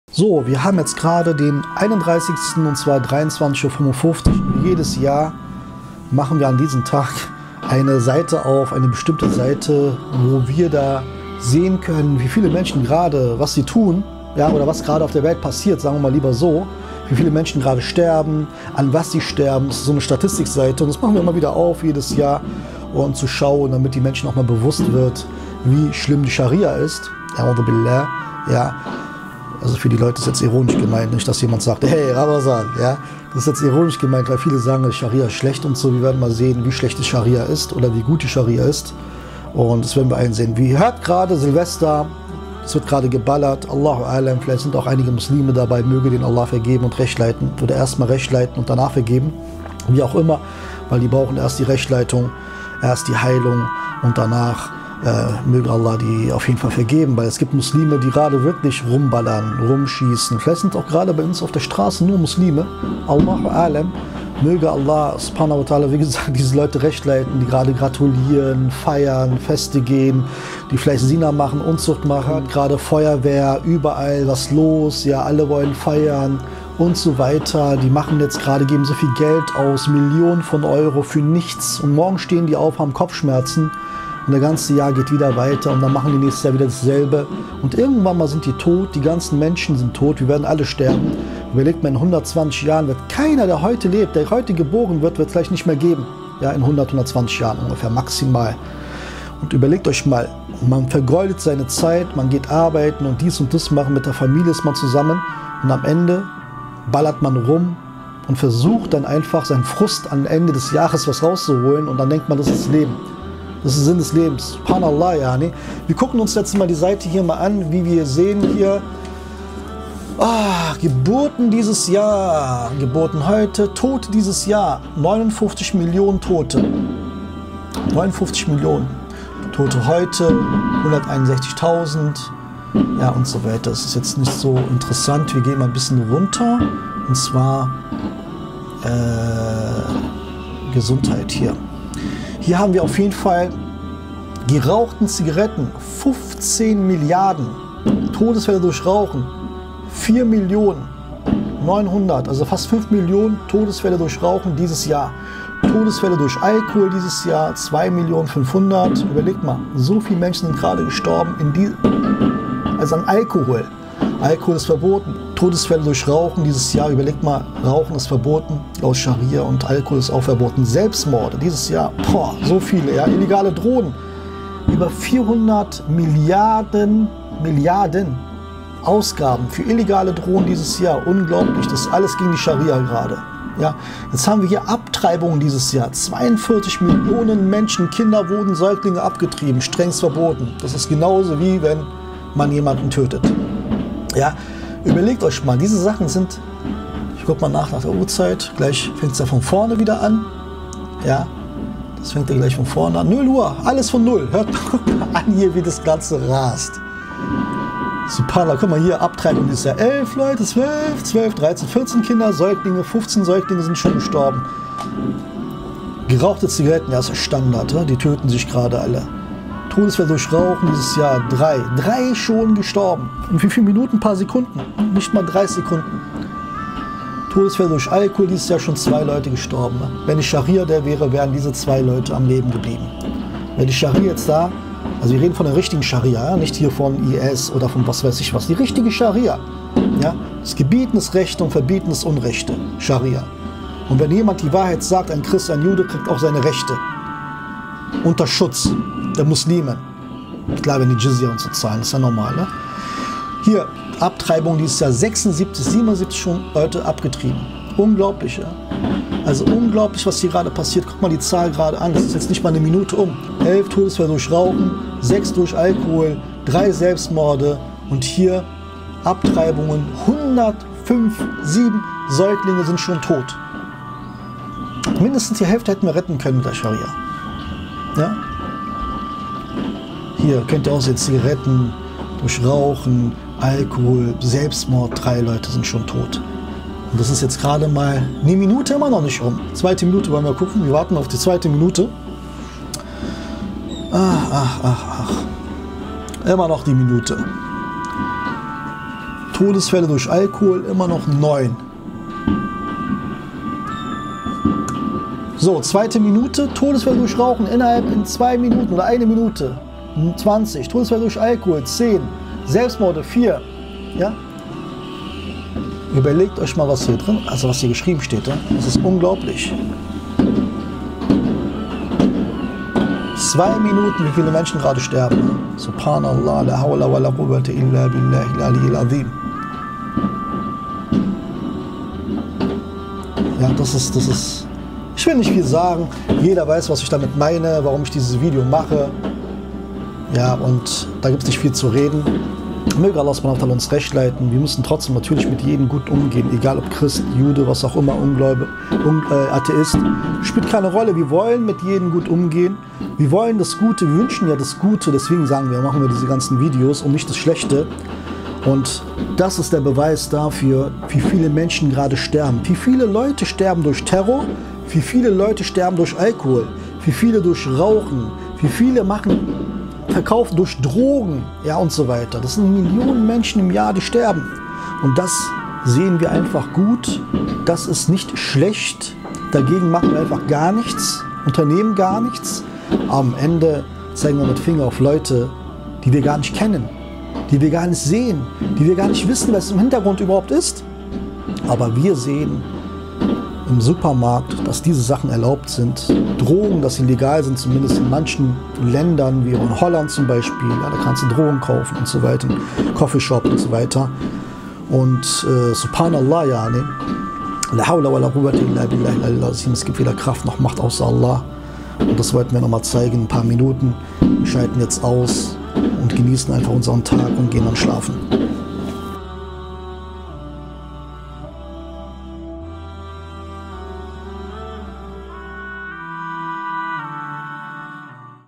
2019 - Sharia hätte Leben gerettet!
Manche Meschen wissen nicht, dass die Sharia viel mehr ist als das, was man in den Medien hört.
Wir zeigen euch hier an Hand ein zwei Beispielen, dass Sharia Leben rettet.
Möge Allah der Erhabene uns Rechtleiten und uns zu besseren Muslimen machen und uns vor dem Übel in der Dunya schützen und uns das Beste in dieser und der nächsten Welt geben, Amin.